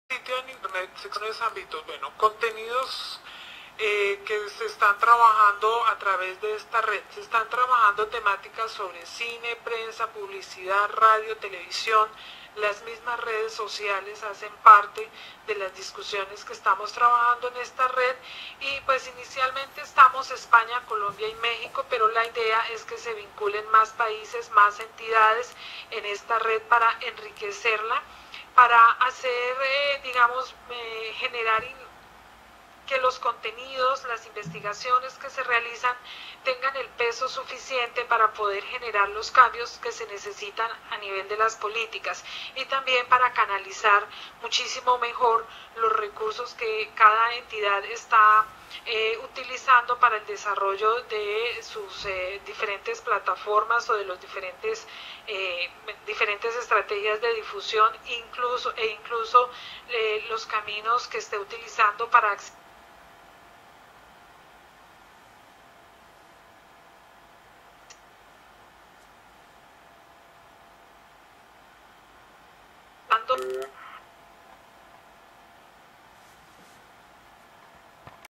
BIREDIAL'12: ponencia Red de archivos sonoros, audiovisuales y multimedia con proyección iberoameri
Título: BIREDIAL'12: ponencia Red de archivos sonoros, audiovisuales y multimedia con proyección iberoamericana (Colombia, México, España)

Descripción: Presentación ponencia en BIREDIAL'12 (Universidad del Norte, Barranquilla-Colombia, 13-16 noviembre 2012) a cargo Dora Brausin, Coordinadora de la Fonoteca de Radio Nacional de Colombia, sobre el tema Red de archivos sonoros, audiovisuales y multimedia con proyección iberoamericana (Colombia, México, España). Más información en el Blog Audiovisualnet.com y en Redocom 2.0

Autor: Alfonso López Yepes

Fecha: 15/11/2012

Materia: Ciencias de la Información

Submateria: Comunicación audiovisual

Editor: Servicio de Documentación Multimedia. Facultad de Ciencias de la Información, Universidad Complutense de Madrid

Formato: MPEG-2

Fuente: Complumedia-RTVDoc

Idioma: Español

Derechos: Servicio de Documentación Multimedia. Facultad de Ciencias de la Información, Universidad Complutense de Madrid